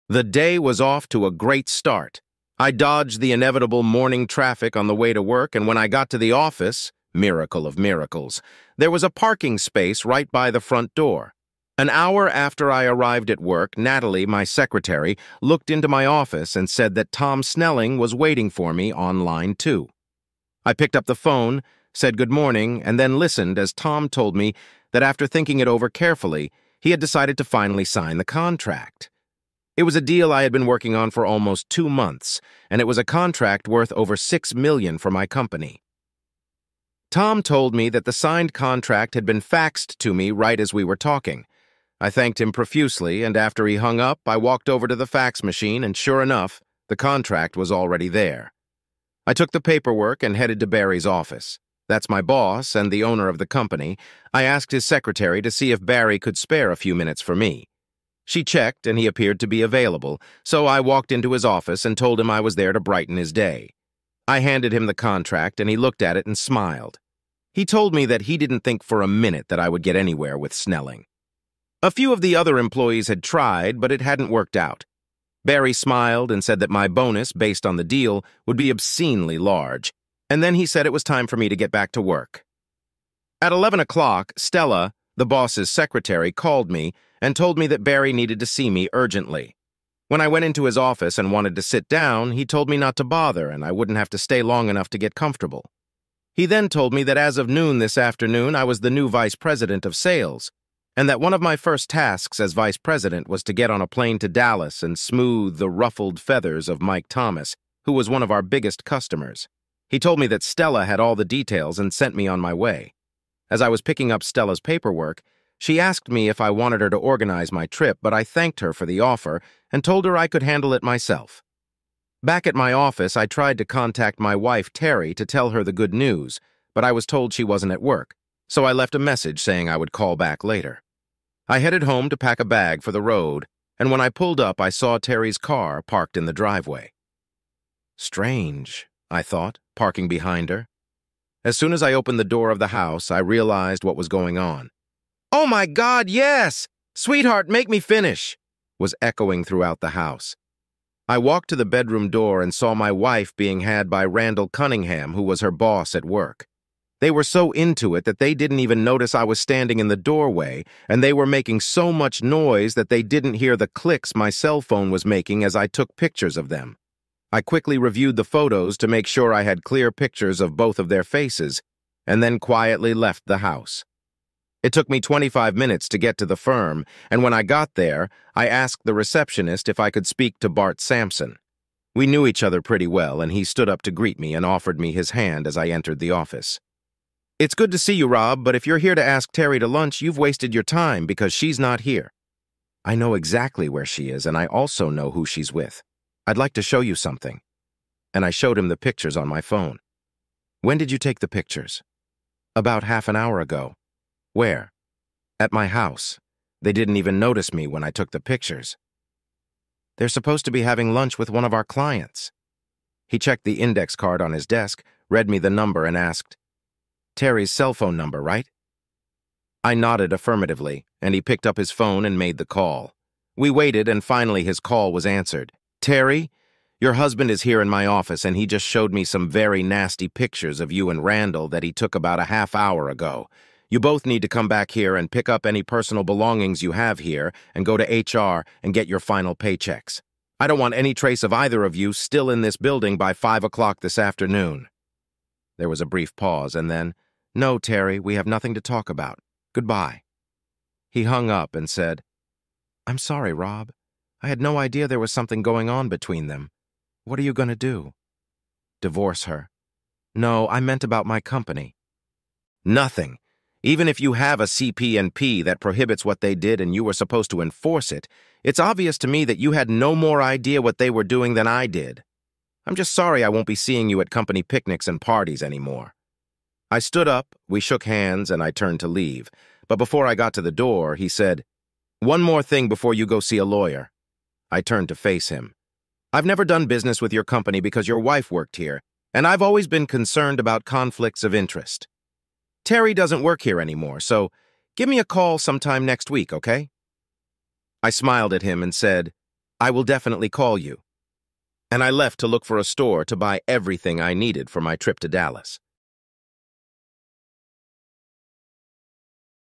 💔 Betrayal Unveiled: Wife's Affair with Boss Shatters Husband's World
In this heart-wrenching video, we dive into a story of shattered trust and devastating betrayal. Witness the unraveling of a once-happy marriage as a husband uncovers his wife's affair with her boss. Prepare to be moved as we explore the emotional aftermath and offer insights into healing and rebuilding trust. This powerful tale serves as a cautionary reminder of the fragile nature of relationships and the importance of open communication. Don't miss this gripping account that will leave you pondering the boundaries of love and loyalty.